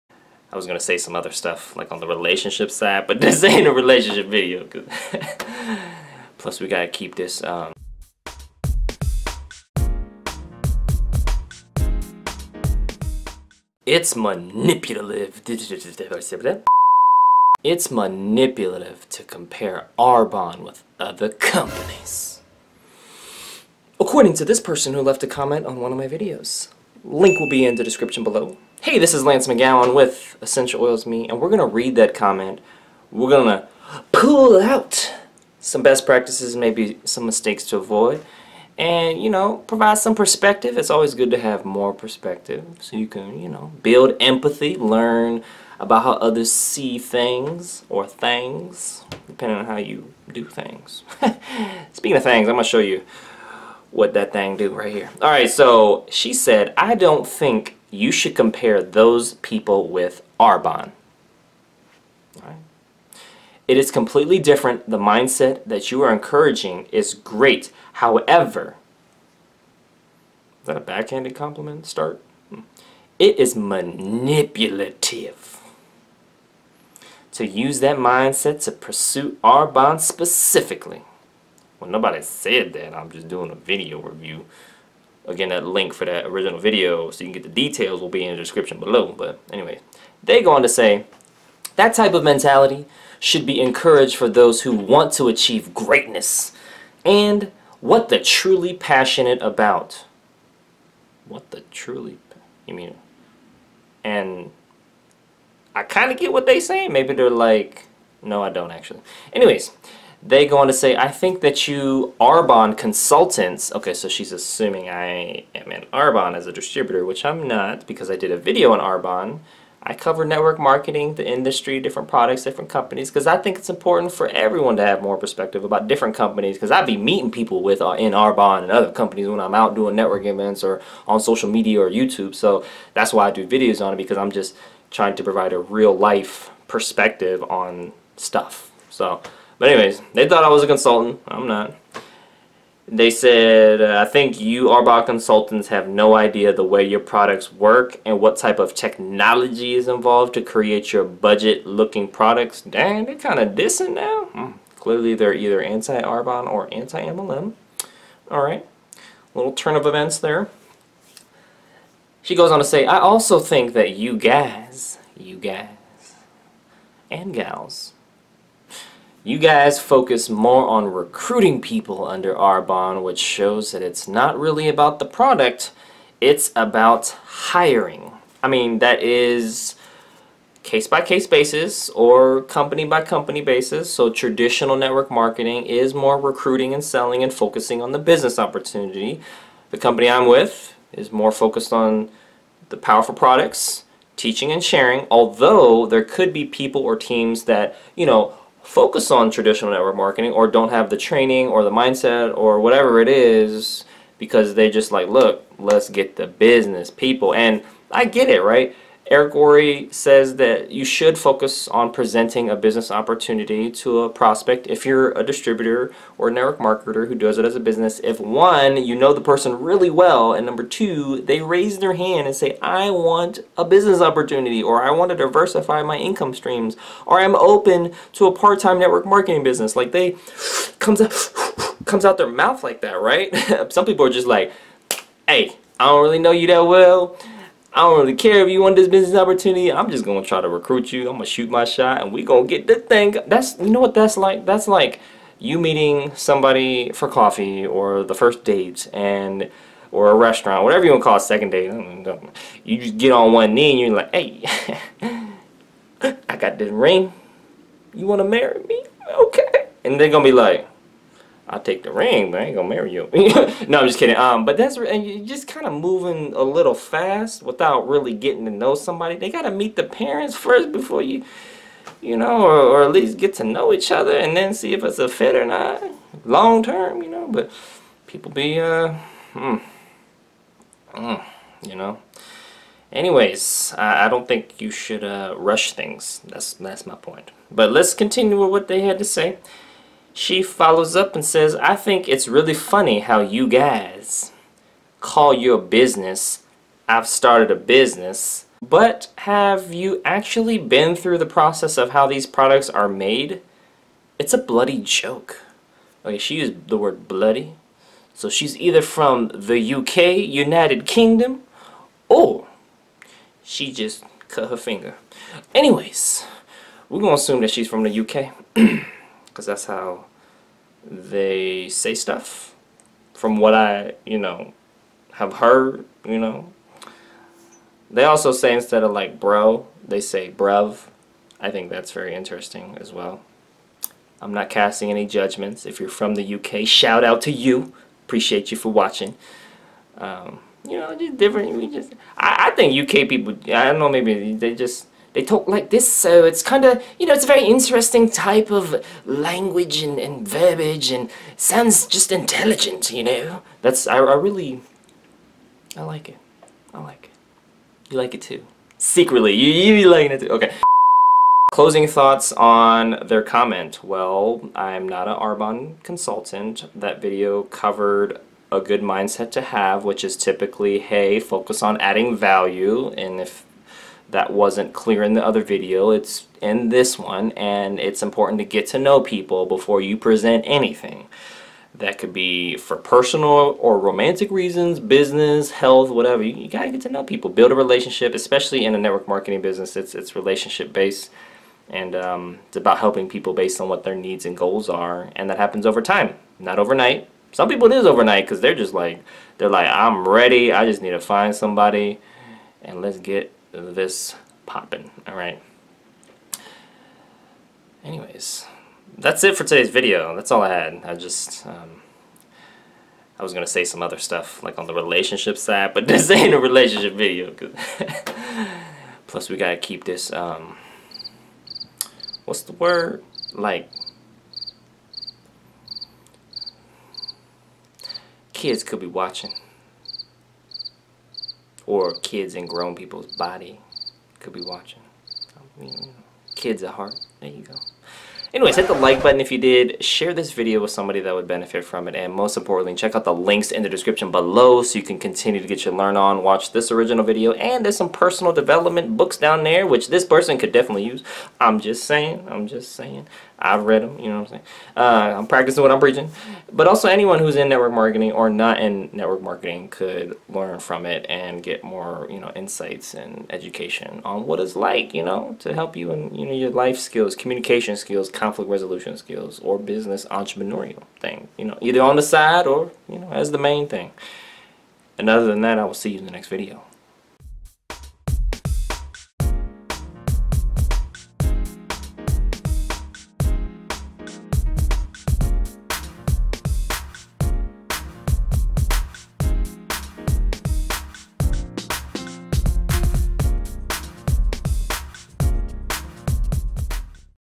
Manipulative To Compare Arbonne With Other Companies
Her: I don’t think you should compare those people with Arbonne. It is completely different the mindset that you are encouraging is great how ever it is manipulative to use that mindset to pursue Arbonne specifically.

That type of mentality should be encouraged for those who want to achieve greatness and what the truly passionate about. I think that you airborne consultants have no idea the way your products work and what type of technology is involved to create your budget looking products.

I also think that you guys focus more on recruiting people under Arbonne which shows that it’s not really about the product it’s about “hiring”. I think it’s really funny how you guys call your business “I’ve started a business” but have you actually been through the process of how these products are made it’s a bloody joke.

d.) Jordan Belfort, Way Of The Wolf

e.) How To Solve Your People Problems

This section is intentionally left blank (I've always wanted to say that)

COMMENT ► On what you liked most about this video!